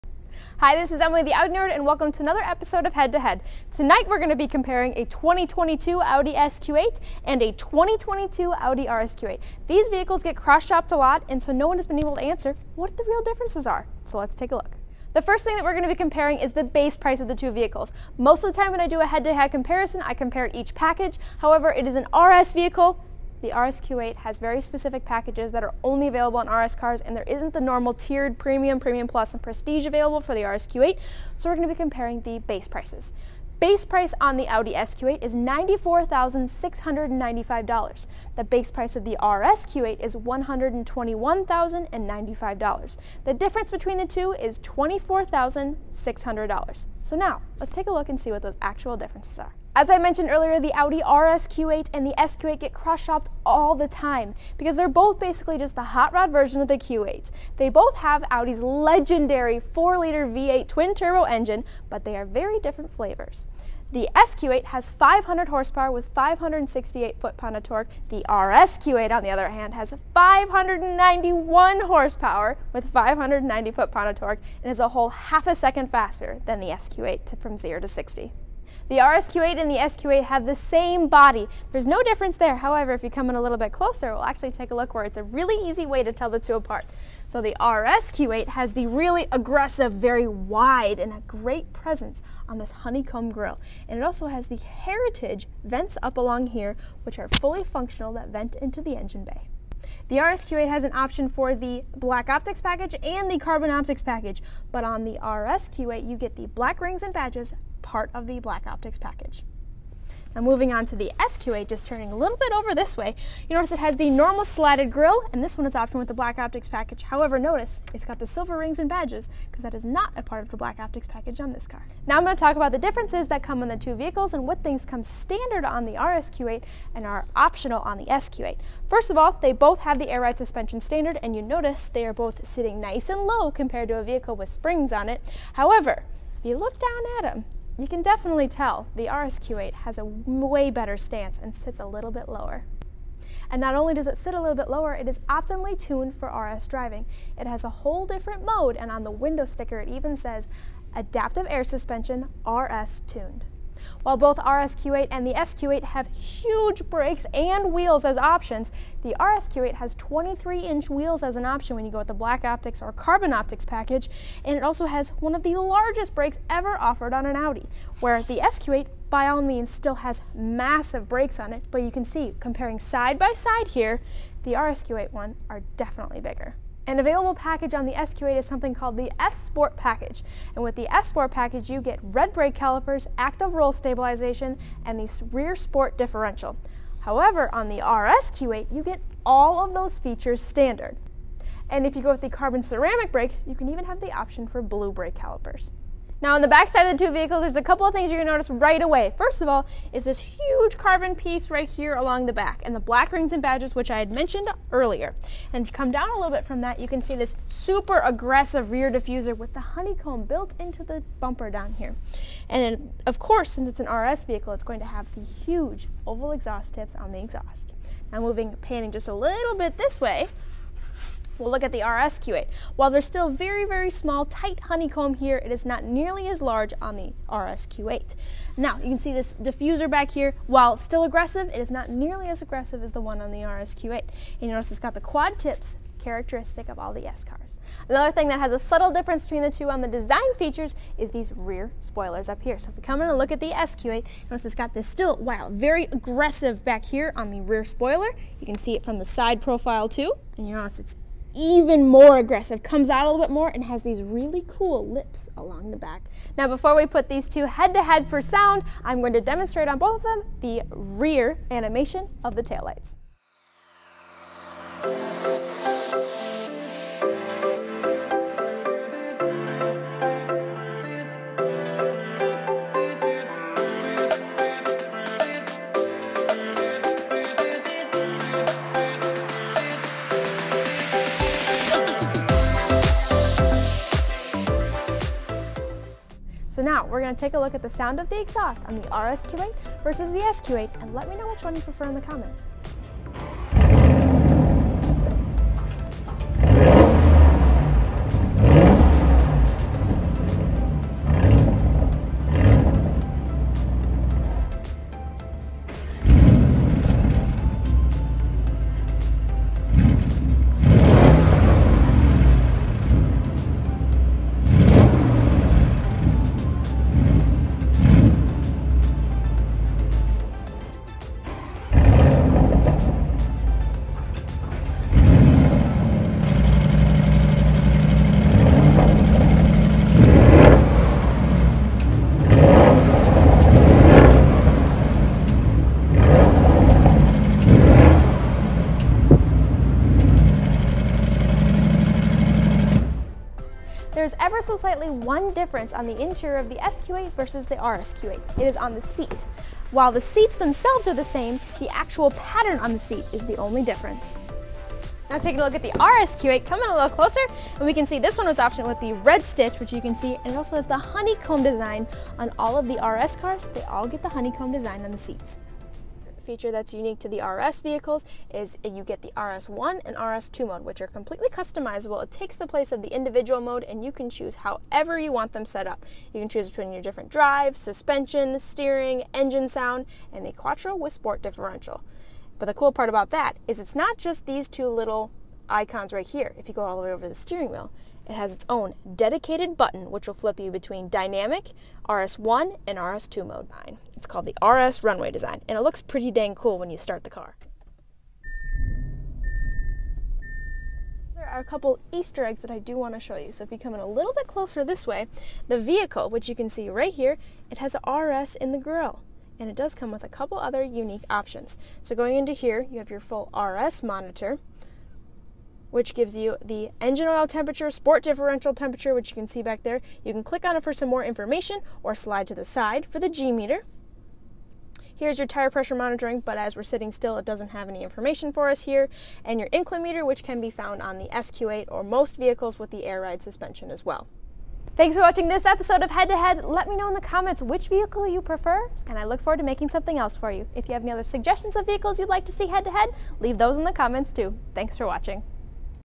Head to Head! Audi SQ8 vs Audi RSQ8
Both the Audi SQ8 and the RSQ8 (or the baby Lamborghini urus) have Audi’s legendary 4.0L V8 twin turbo engine. But they are very different flavors. I put these two cars head to head and compare the differences and go over the special RS features only on the RSQ8.

So what one do you prefer?